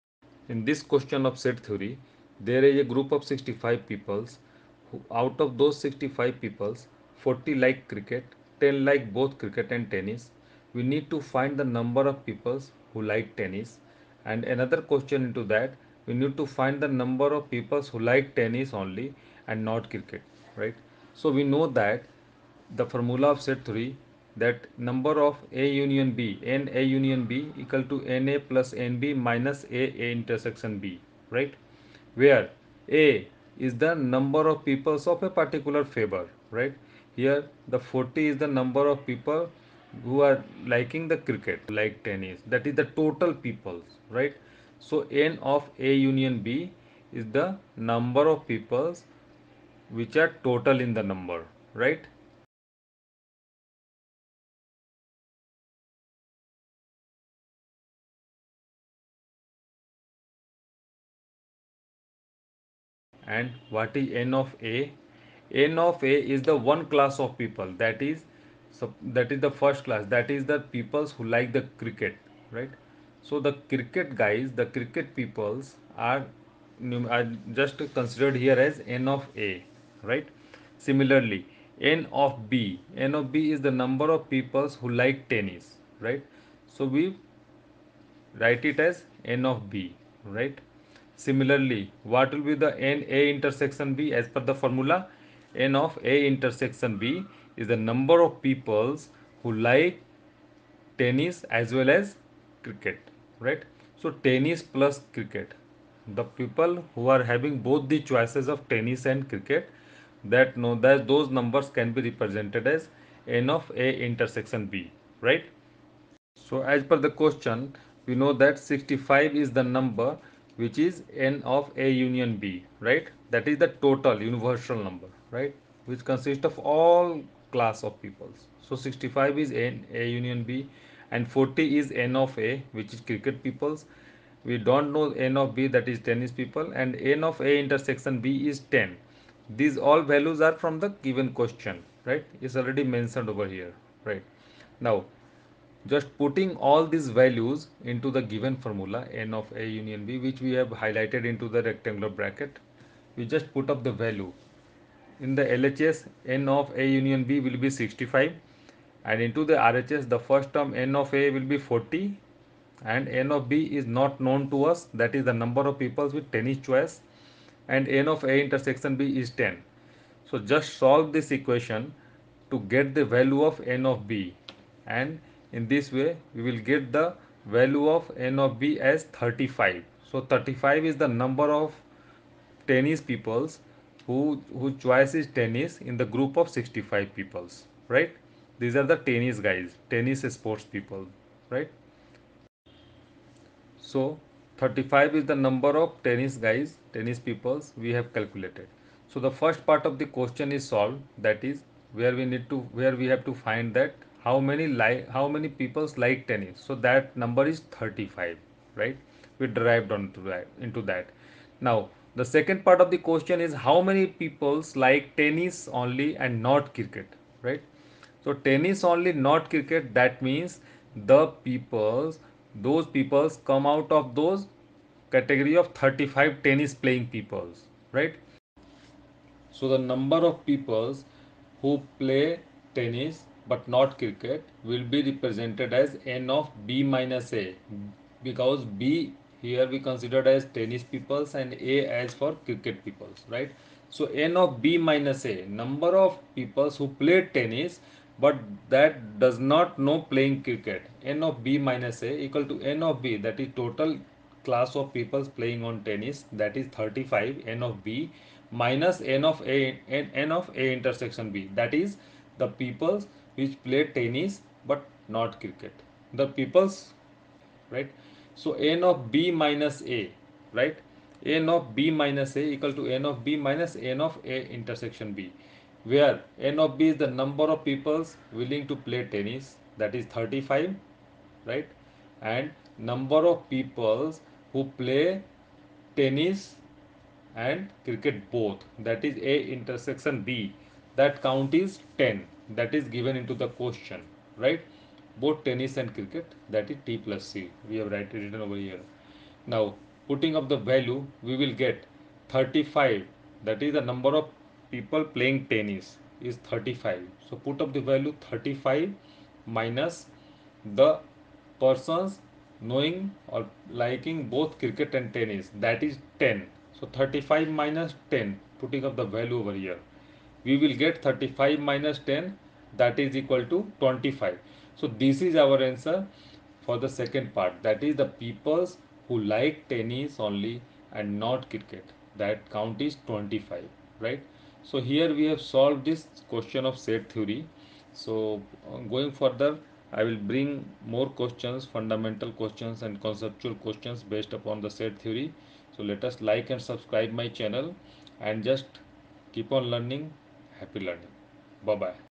Of 65 people, 40 like cricket, 10 like both C,T How many 1) like Tennis2)Tennis only and not cricket
set theory
class 11
Question:
out Of 65 people, 40 like cricket, 10 like both Cricket and Tennis. How many 1) like Tennis only 2) How many like Tennis only and not cricket?